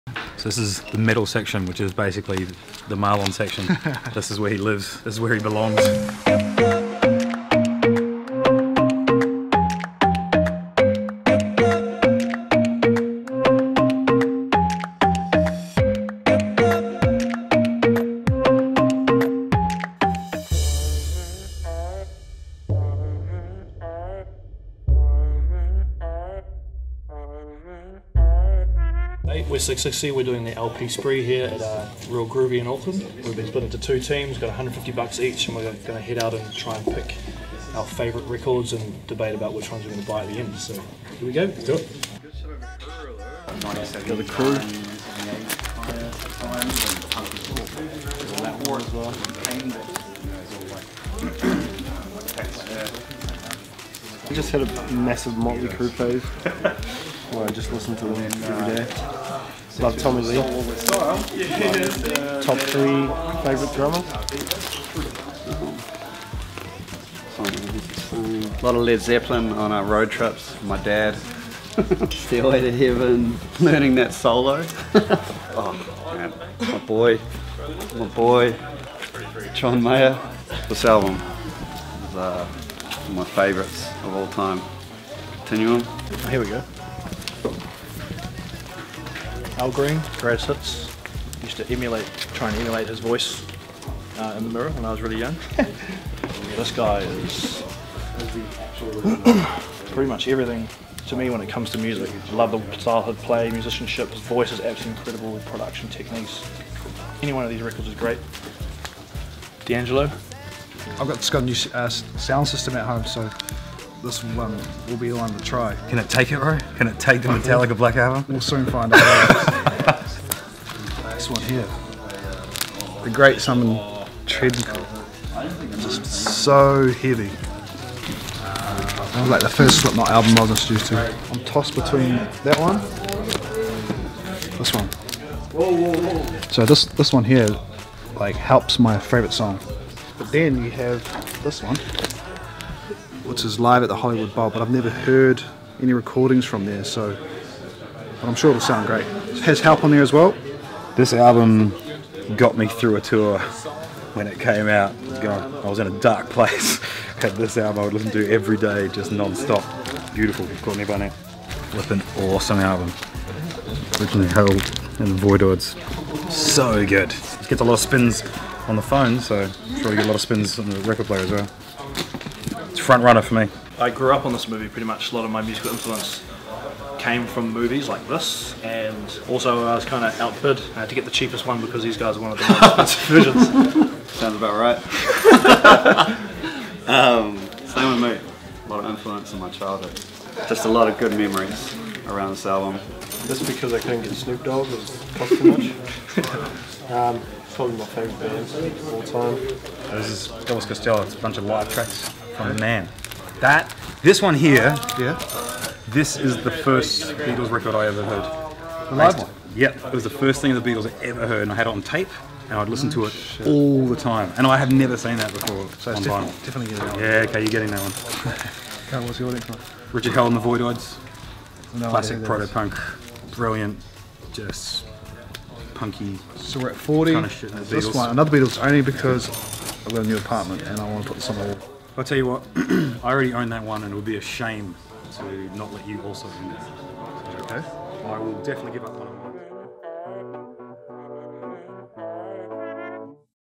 It's Six60 vs. Six60... Who's Gonna Win? | LP Spree
One Band. Two Teams with $150 each.  It's a battle of the LPs.  Who's gonna win?